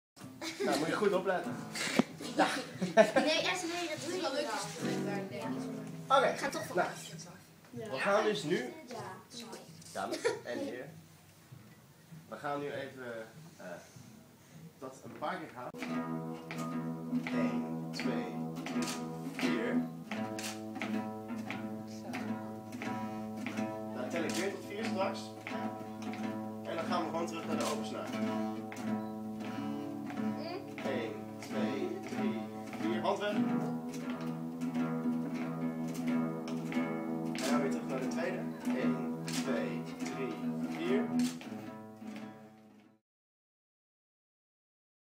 Leerlingen van de de Carrousel in Ankum krijgen muziekles
Ewoud van popmuziekschool Rhythm & Music geeft gitaarles aan leerlingen groep 6 en 7.